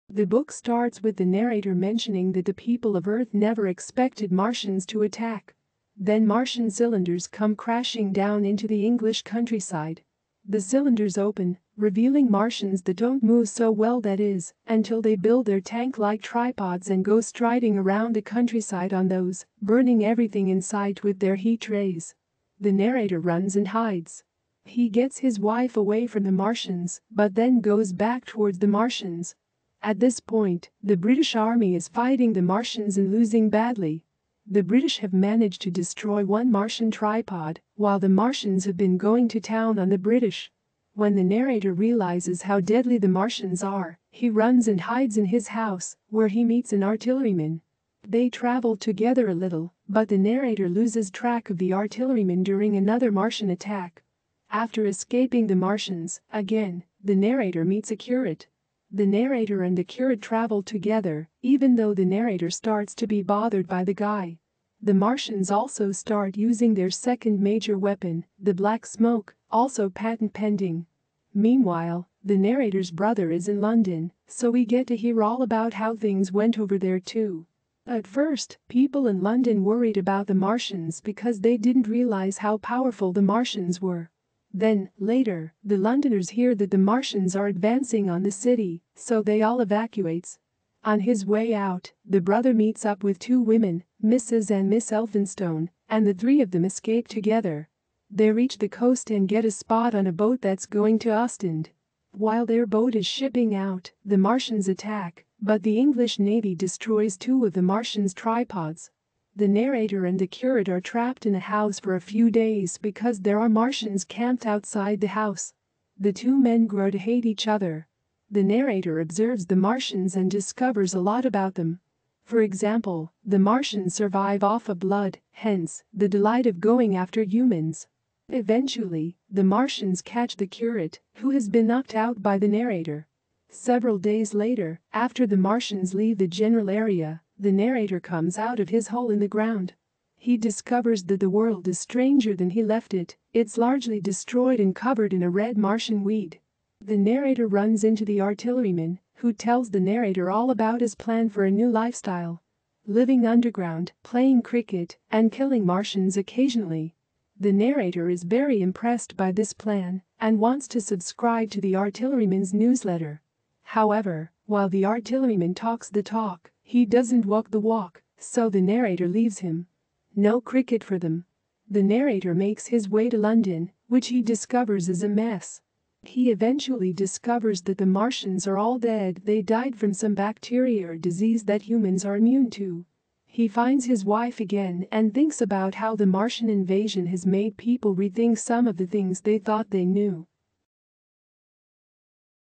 Aliens land on earth and start destroying all life
The War of the Worlds Summary

What is The War of the Worlds book about?

The War of the Worlds is a science fiction novel written by H.G. Wells in 1898.
Plot Summary